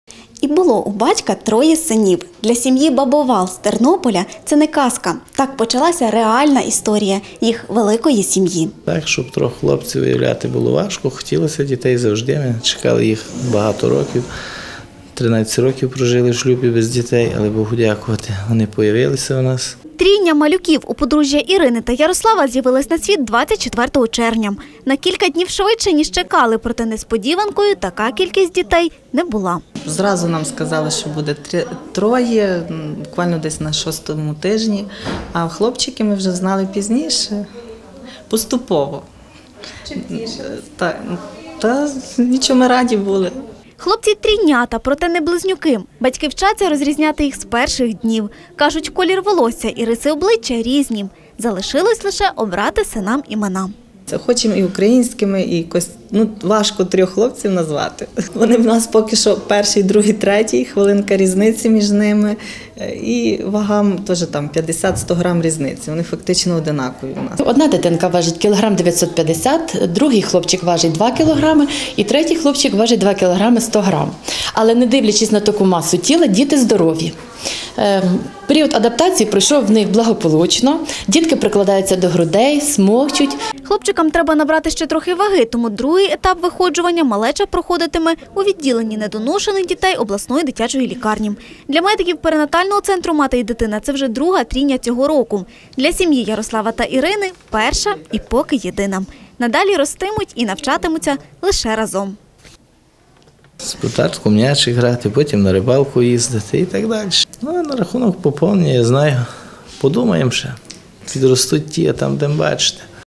У Тернопільському обласному перинатальному центрі " Мати і дитина" народилась трійня
Потрійне щастя після довгих років чекання. У Тернопільському обласному перинатальному центрі " Мати і дитина"  народилась трійня. Усі - хлопчики, проте не близнюки. Першим малечу побачив тато. Своєю радістю з "Провінційними вістями" поділились обоє батьків.